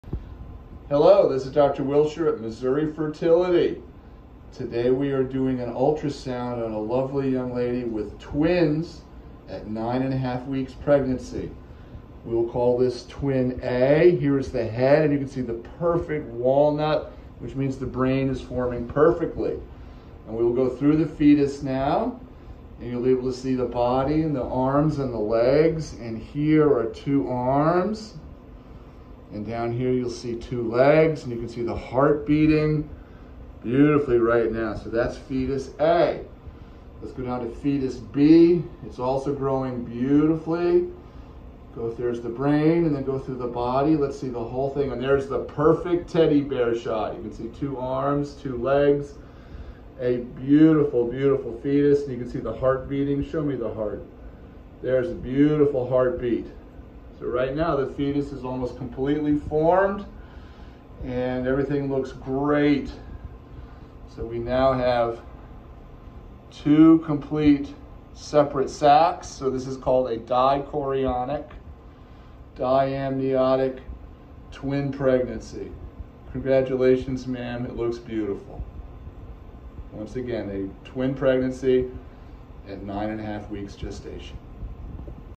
Ultrasound of Twin Pregnancy at 9 1/2 weeks
An ultrasound of 9 1/2 week twin pregnancy.